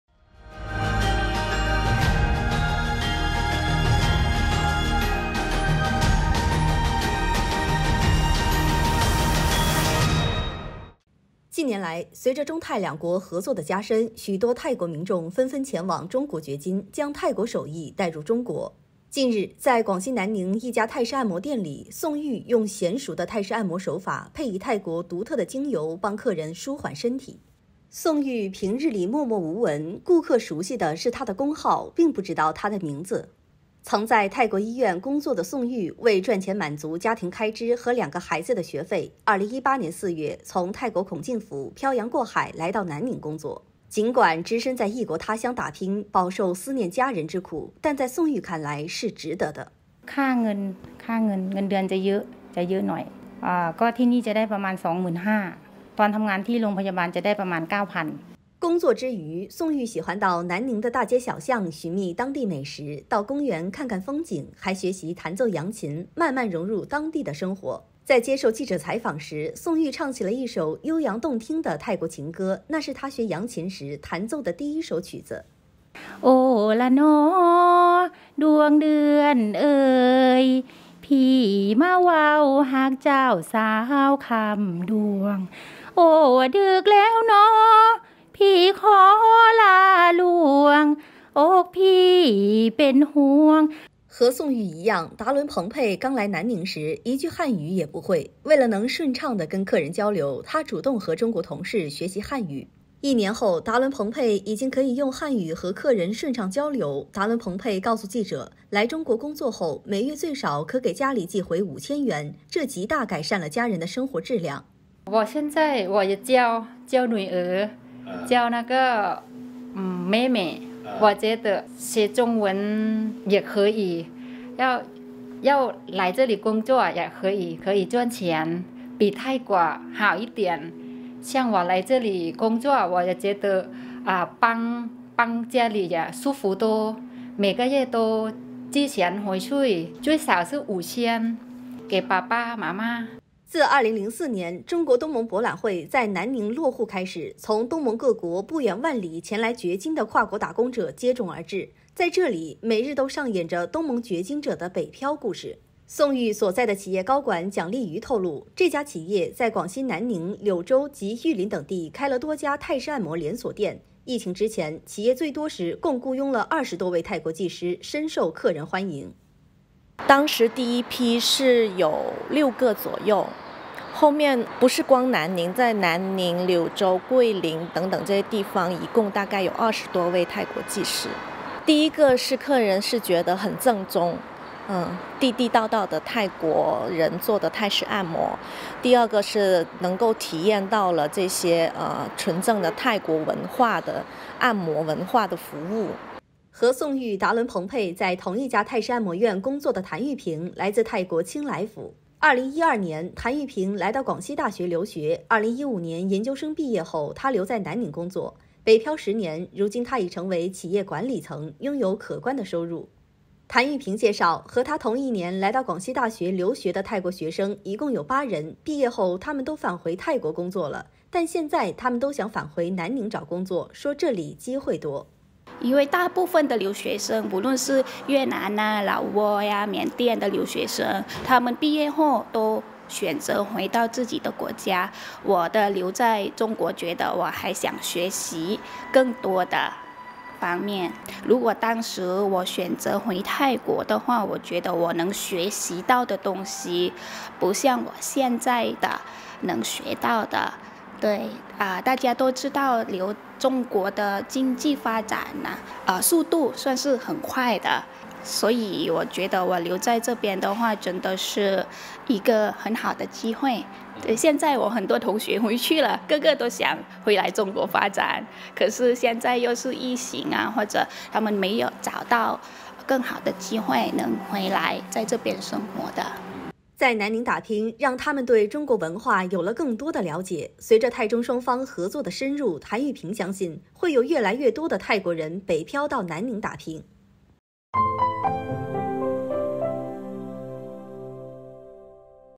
泰国按摩师逐梦南宁：学习中文 融入当地
近年来，随着中泰两国合作的加深，许多泰国民众纷纷前往中国“掘金”，将泰国手艺带入中国。近日，在广西南宁一家泰式按摩店里，宋玉用娴熟的泰式按摩手法配以泰国独特的精油帮客人舒缓身体。

宋玉平日里默默无闻，顾客熟悉的是她的工号，并不知道她的名字。曾在泰国医院工作的宋玉，为赚钱满足家庭开支和两个孩子的学费，2018年4月，从泰国孔敬府漂洋过海来到南宁工作。尽管只身在异国他乡打拼，饱受思念家人之苦，但在宋玉看来是值得的。

工作之余，宋玉喜欢到南宁的大街小巷寻觅当地美食，到公园看看风景，还学习弹奏扬琴，慢慢融入当地的生活。在接受记者采访时，宋玉唱起了一首悠扬动听的泰国情歌，那是她学扬琴时弹奏的第一首曲子。

和宋玉一样，达伦·蓬佩刚来南宁时，一句汉语也不会。为了能顺畅地跟客人交流，她主动和中国同事学习汉语。一年后，达伦·蓬佩已经可以用汉语和客人顺畅交流。达伦·蓬佩告诉记者，来中国工作后，每月最少可给家里寄回5000元，这极大改善了家人的生活质量。

自2004年中国—东盟博览会在南宁“落户”开始，从东盟各国不远万里前来“掘金”的跨国打工者接踵而至，在这里，每日都上演着东盟“掘金”者的“北漂”故事。宋玉所在的企业高管蒋丽瑜透露，这家企业在广西南宁、柳州及玉林等地开了多家泰式按摩连锁店，疫情之前，企业最多时共雇佣了20多位泰国技师，深受客人欢迎。

和宋玉、达伦·蓬佩在同一家泰式按摩院工作的谭玉萍来自泰国清莱府。2012年，谭玉萍来到广西大学留学，2015年研究生毕业后，她留在南宁工作。“北漂”十年，如今她已成为企业管理层，拥有可观的收入。谭玉萍介绍，和她同一年来到广西大学留学的泰国学生一共有八人，毕业后他们都返回泰国工作了。但现在他们都想返回南宁找工作，说这里机会多。

在南宁打拼，让她们对中国文化有了更多的了解。随着泰中双方合作的深入，谭玉萍相信，会有越来越多的泰国人“北漂”到南宁打拼。（陈沿佑 蒋雪林 陈秋霞）